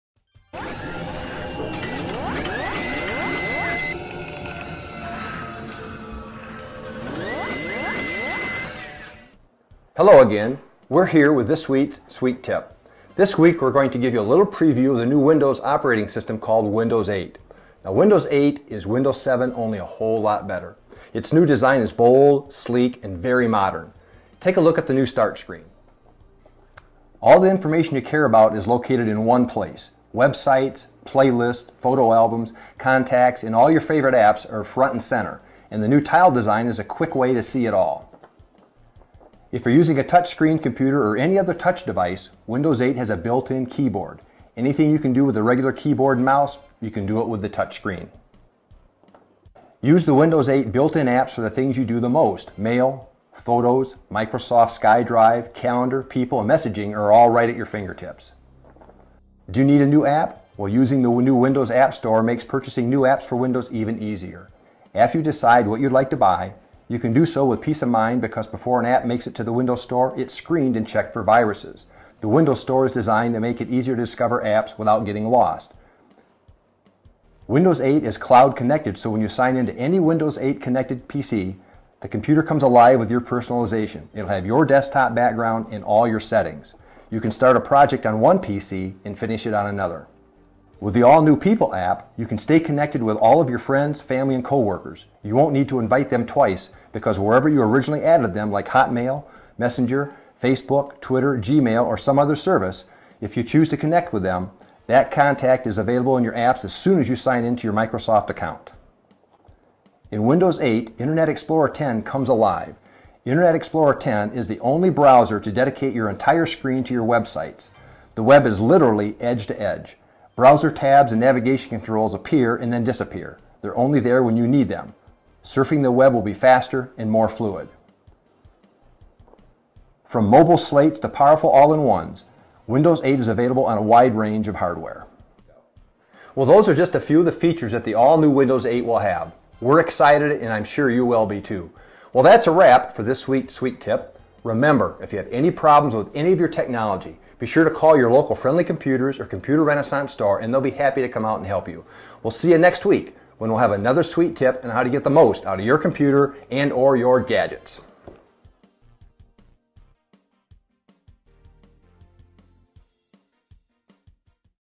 Friendly Computers & Computer Renaissance Sweet Tips & Tricks: Windows 8 Consumer Preview Tips
Windows 8 is the latest operating system by Microsoft. With several features imported from it's smart phones and Windows 7, it makes for a great user experience.